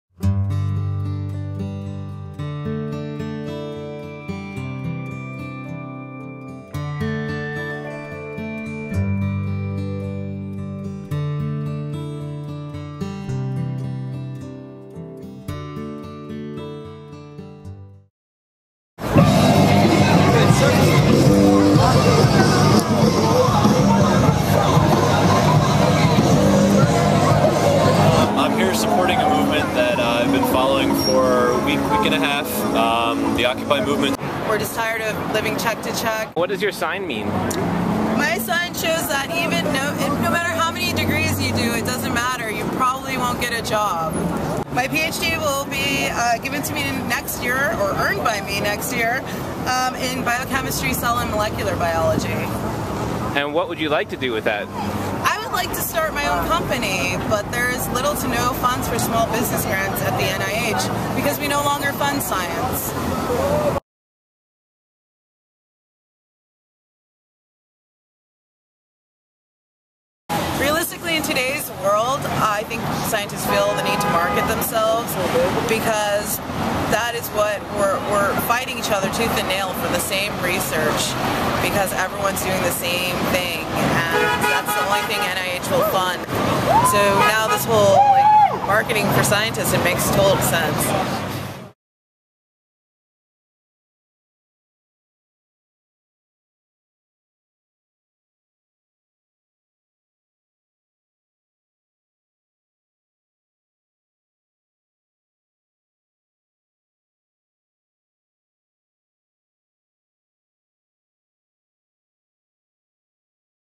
Scientists Join Occupy Movement
Marketing for Scientists: Scientists join the Occupy movement, protesting diminished funding and opportunities.